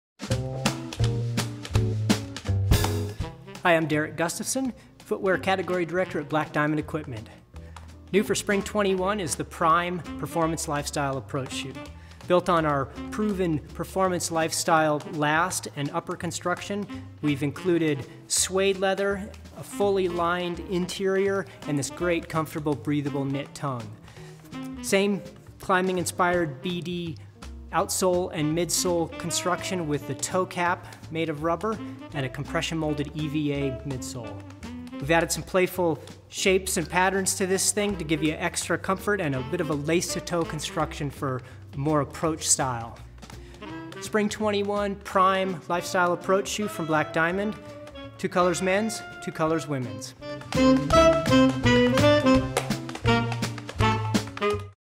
[블랙다이아몬드] 프라임 어프로치 슈즈
내구성과 편안함, 스타일을 모두 갖춘 프라임 어프로치 슈즈는 고급스런 스웨이드 갑피를 적용하여 편안한 착용감은 물론 뛰어난 기후 저항력을 갖추고 있습니다.